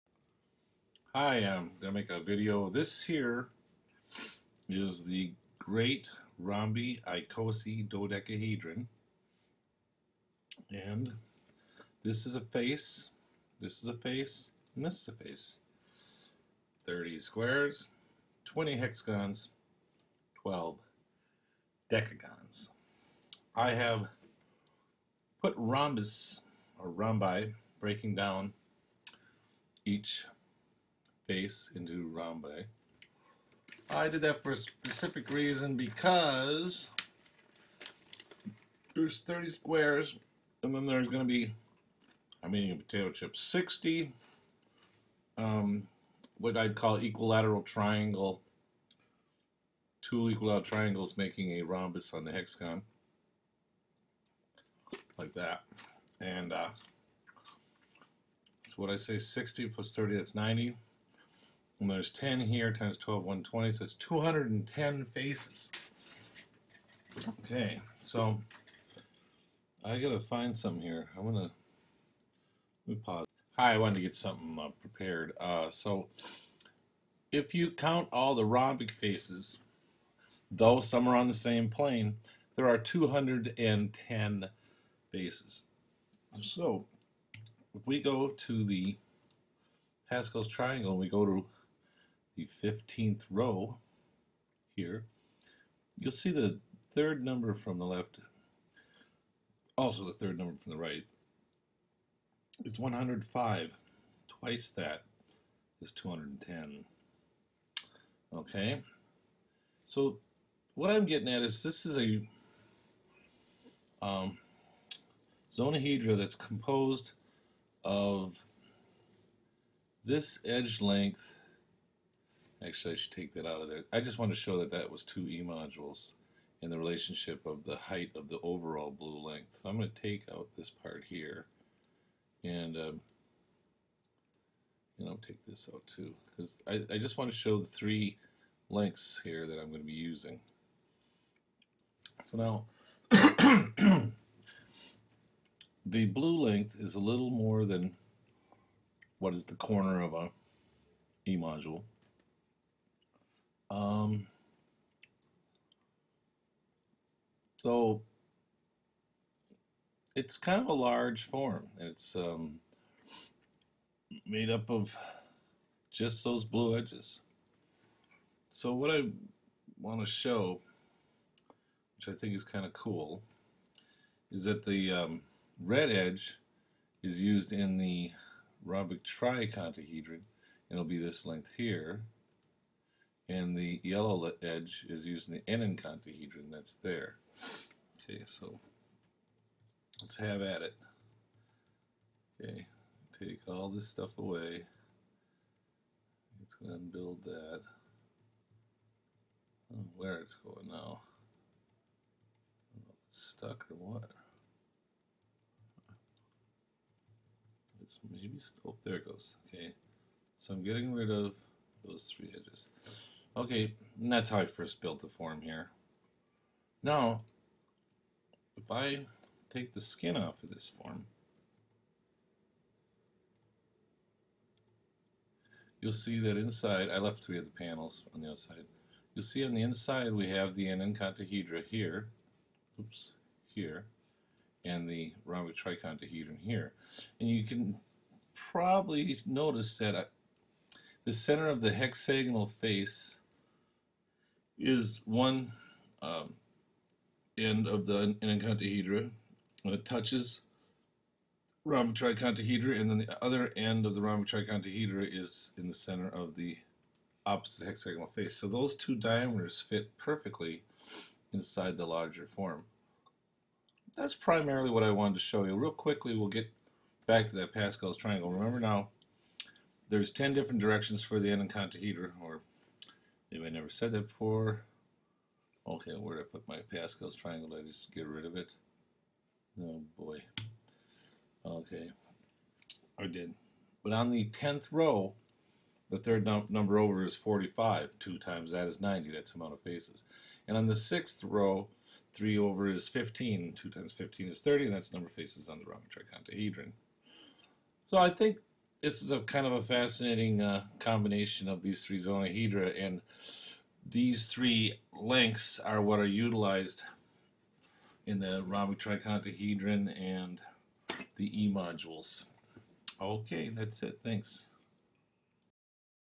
Great Rhombicosidodecahedron
I show a relationship between the great rhombicosidodecahedronm the enneacontahedron and the rhombic triacontahedron.